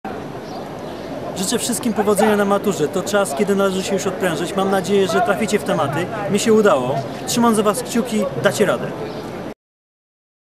Matura 2011 - Dasz Radę!
Poseł Michał Jaros życzy powodzenia tegorocznym maturzystom. Dacie radę!